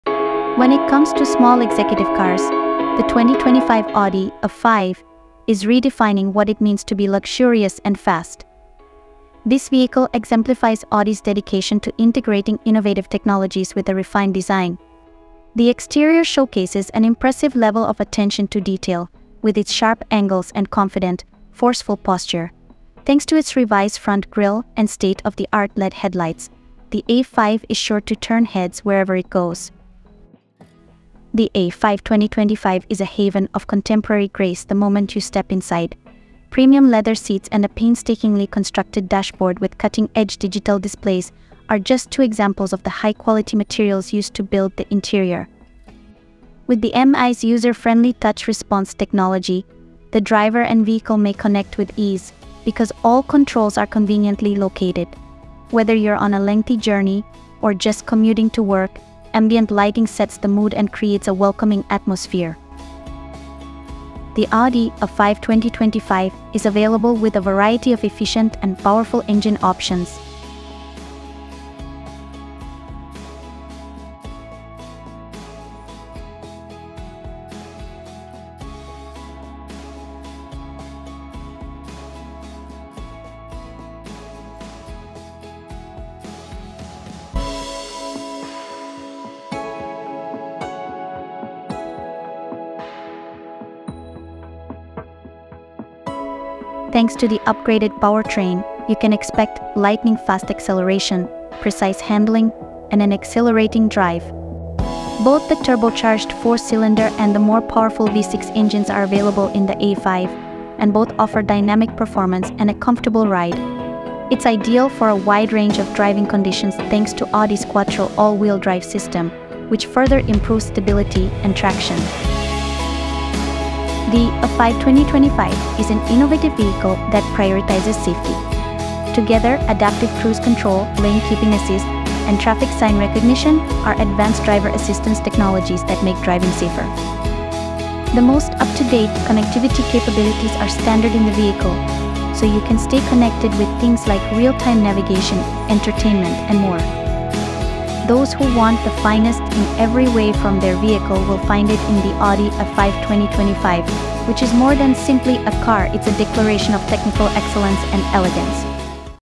First Look: 2025 Audi A5 – The Ultimate Driving Experience!
When it comes to small executive cars, the 2025 Audi A5 is redefining what it means to be luxurious and fast. This vehicle exemplifies Audi's dedication to integrating innovative technologies with a refined design. The exterior showcases an impressive level of attention to detail, with its sharp angles and confident, forceful posture. Thanks to its revised front grille and state-of-the-art LED headlights, the A5 is sure to turn heads wherever it goes.

The A5 2025 is a haven of contemporary grace the moment you step inside. Premium leather seats and a painstakingly constructed dashboard with cutting-edge digital displays are just two examples of the high-quality materials used to build the interior. With the MMI's user-friendly touch response technology, the driver and vehicle may connect with ease because all controls are conveniently located. Whether you're on a lengthy journey or just commuting to work, ambient lighting sets the mood and creates a welcoming atmosphere.

The Audi A5 2025 is available with a variety of efficient and powerful engine options. Thanks to the upgraded powertrain, you can expect lightning-fast acceleration, precise handling, and an exhilarating drive. Both the turbocharged four-cylinder and the more powerful V6 engines are available in the A5, and both offer dynamic performance and a comfortable ride. It's ideal for a wide range of driving conditions thanks to Audi's Quattro all-wheel-drive system, which further improves stability and traction.

The A5 2025 is an innovative vehicle that prioritizes safety. Together, adaptive cruise control, lane-keeping assist, and traffic sign recognition are advanced driver assistance technologies that make driving safer. Those who want the finest in every way from their vehicle will find it in the Audi A5 2025, which is more than simply a car—it's a declaration of technical excellence and elegance.